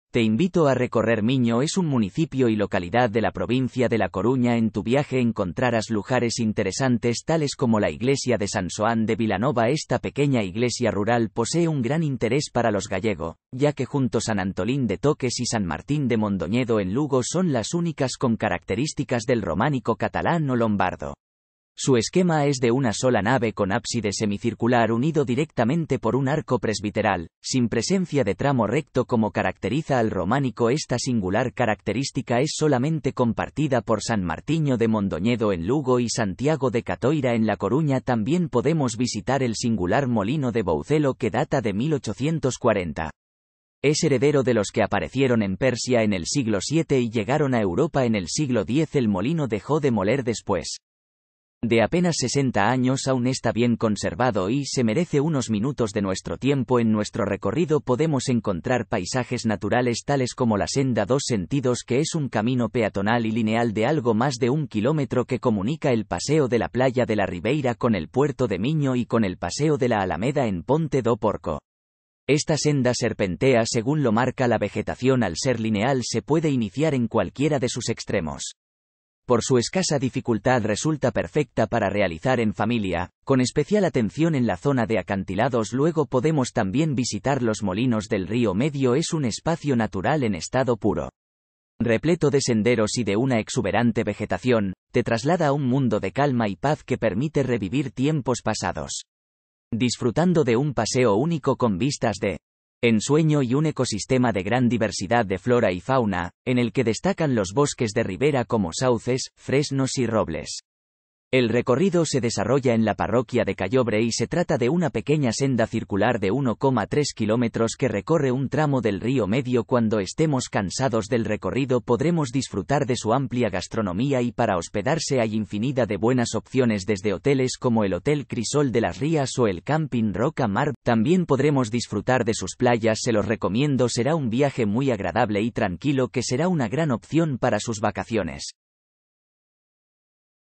¿Qué visitar en Galicia ?te invito a recorrer Miño
es un municipio y localidad de la provincia de La Coruña en tu viaje encontraras lujares interesantes como la Iglesia de San Xoán de Vilanova